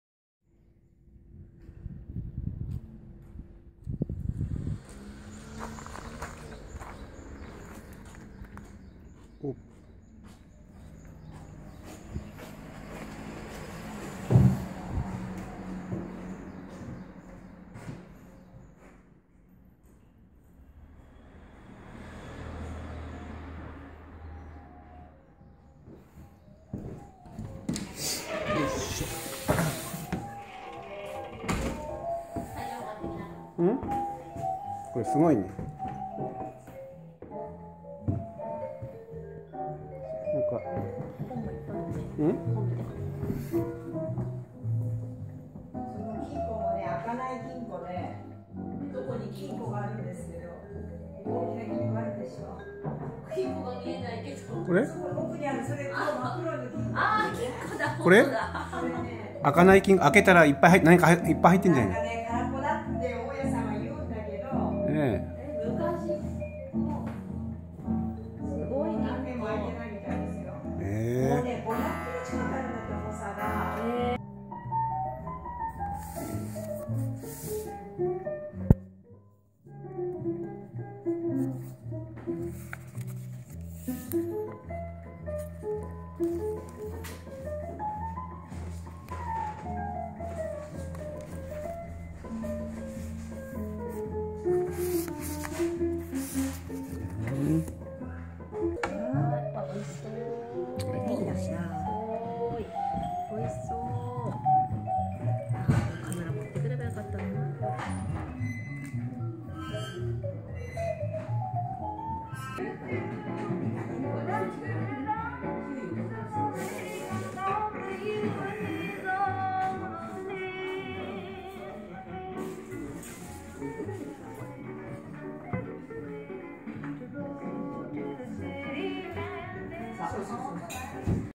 大田原にあるカフェ『as』(アズ)へ行ってきた！
モダンな店内、異空間に居るような雰囲気が魅力的！そしてアイスコーヒーを飲んだけど透明感があるようなスッキリさで美味しかった！また行きます！カフェアズ美味しい！